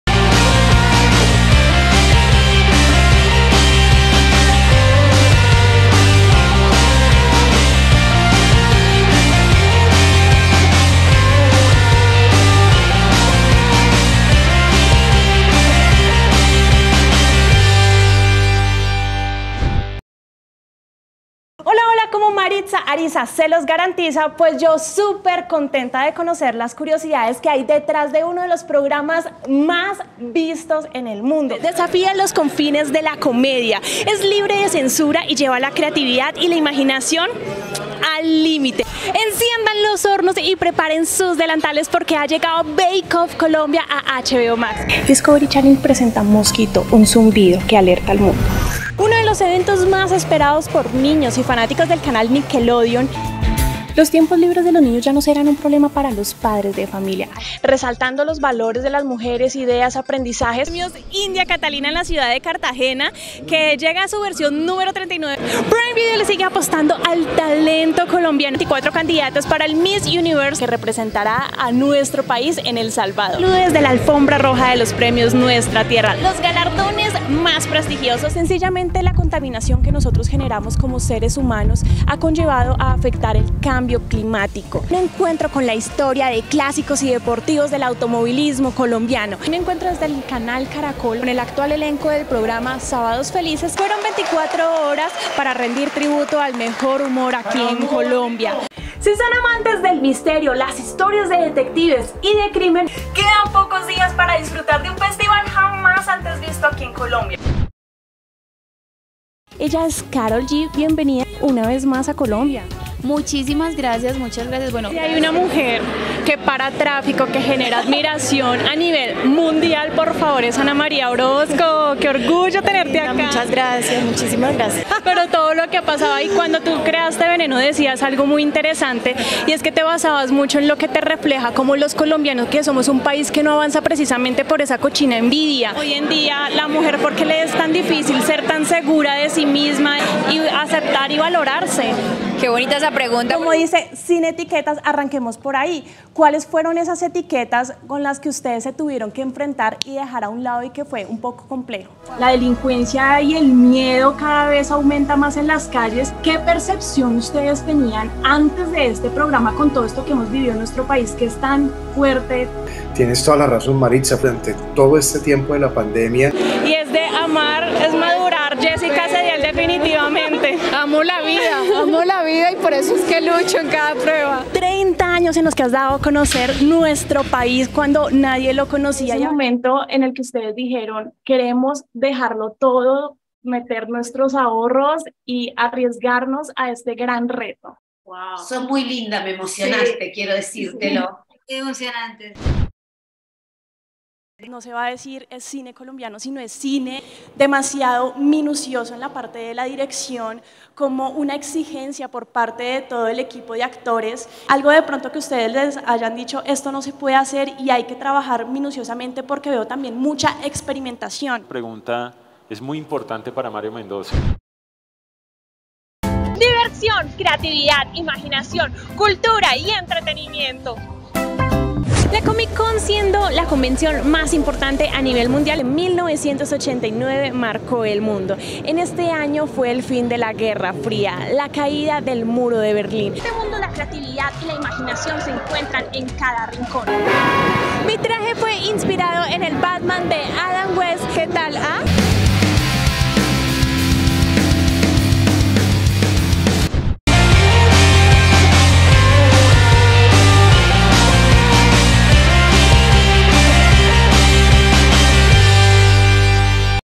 Reel MARITZA ARIZA 🎙️ periodista y creadora de contenido 📽️🎭
✨Un poco de lo que he andado en este maravilloso mundo del periodismo y la creación de contenido. 12 años ejerciendo, 8 como independiente y los que faltan 🙏

Entrevistas a celebridades, cubrimiento de alfombras rojas, recomendados de cine, televisión, teatro, viajes, resúmenes de premiaciones y realities más importantes.

0:00 Resumen
0:19 Presentación
1:48 Entrevistas
3:13 Ruedas de prensa
3:35 Cultura Geek
4:08 Contacto